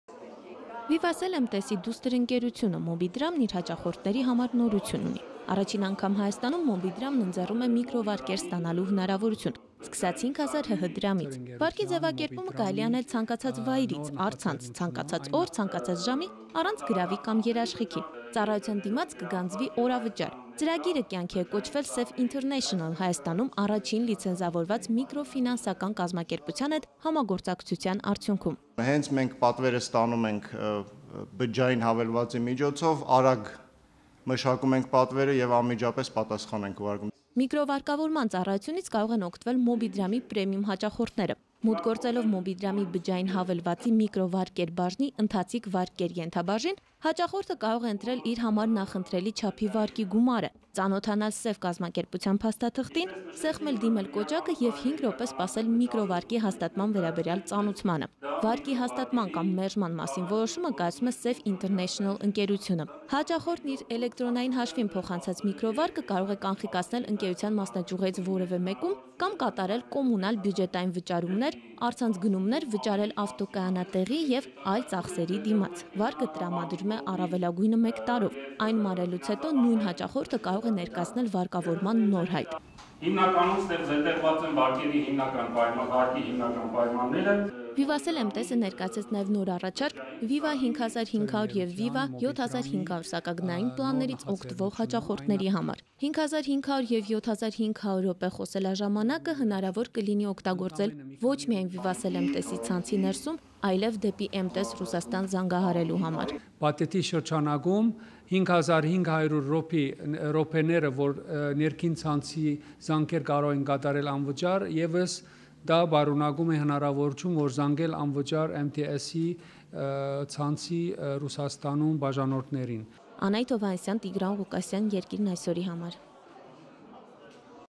ՎիվաՍել-ՄՏՍ-ը ներկայացնում է ՄոբիԴրամի բջջային հավելվածը
ՄոբիԴրամն առաջարկում է առցանց միկրովարկավորում՝ առաջին անգամ Հայաստանում